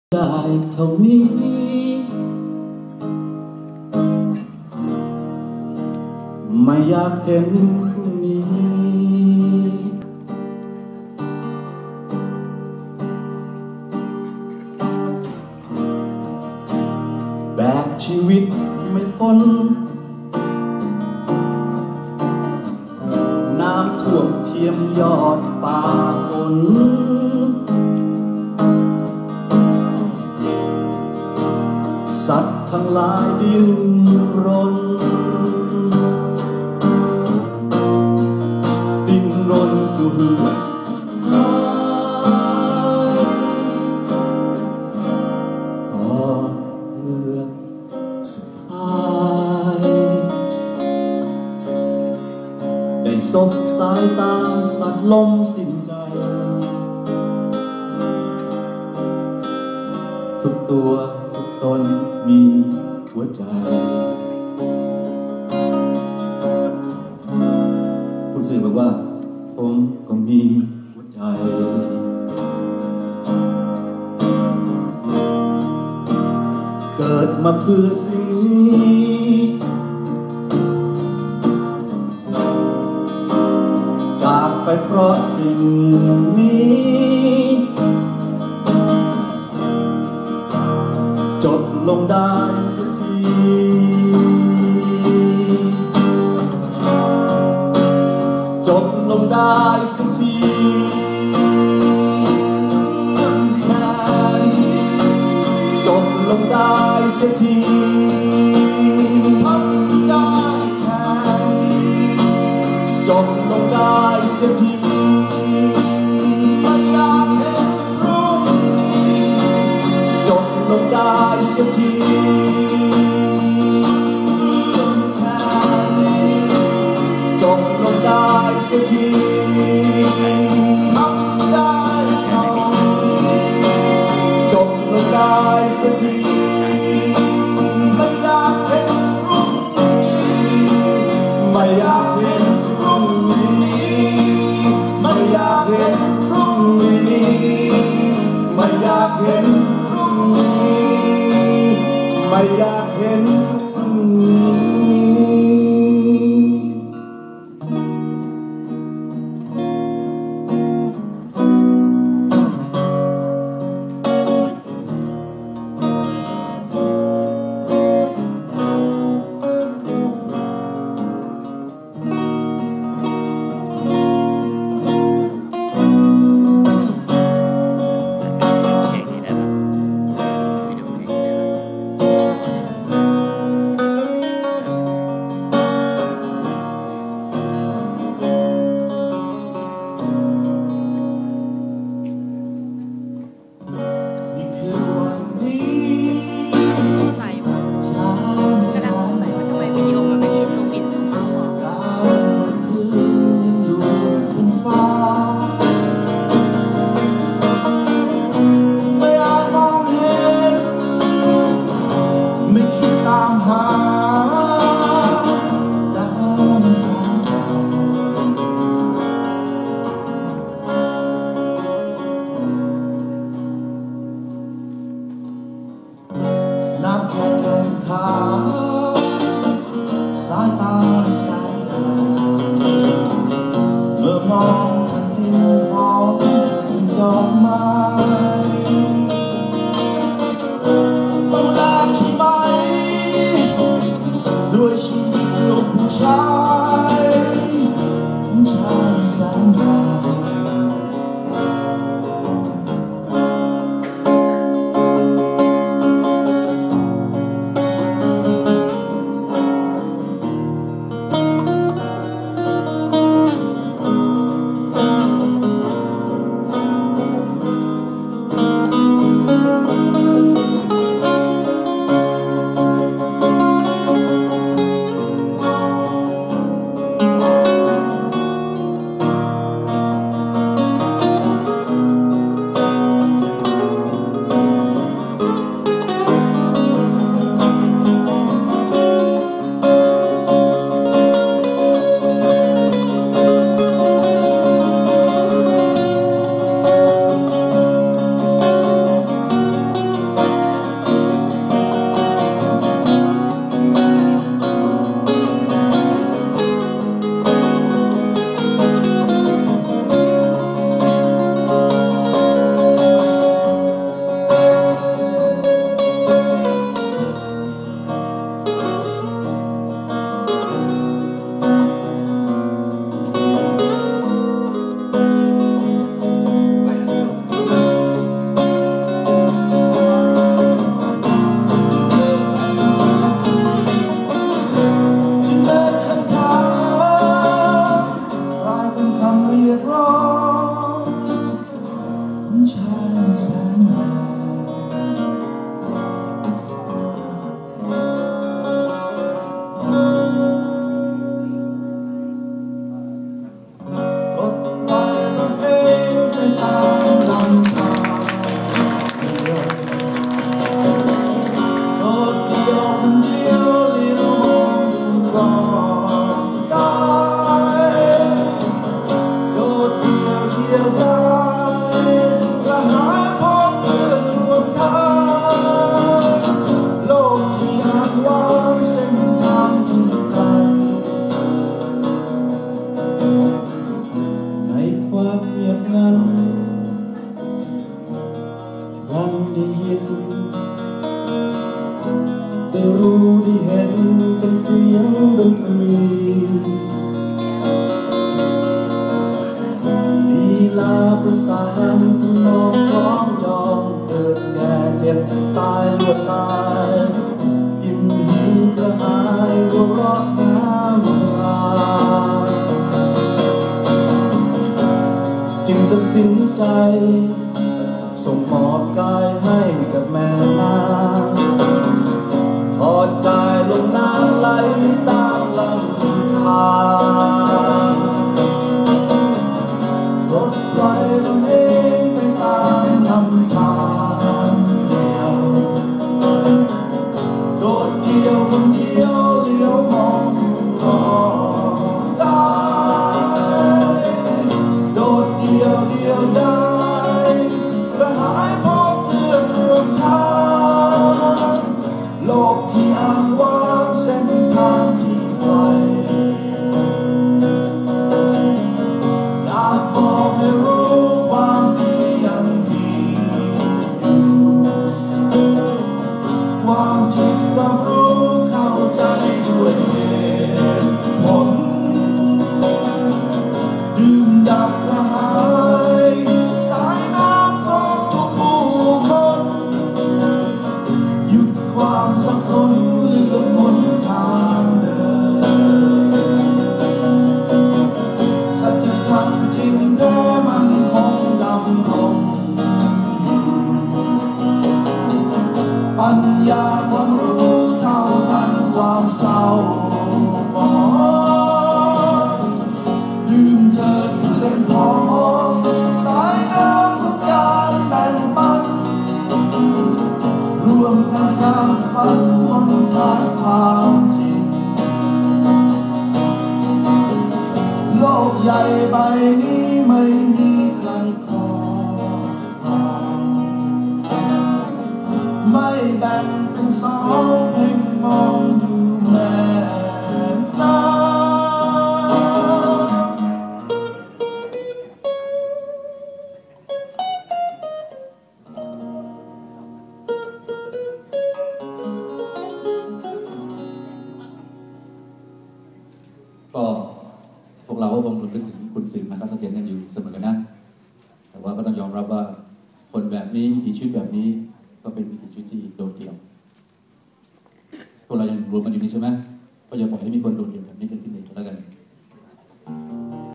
รำลึก 21 ปี สืบ นาคะเสถียร
รำลึก 21 ปี จากป่า สู่เมือง สืบ นาคะเสถียร  ณ หอศิลป์ กรุงเทพฯ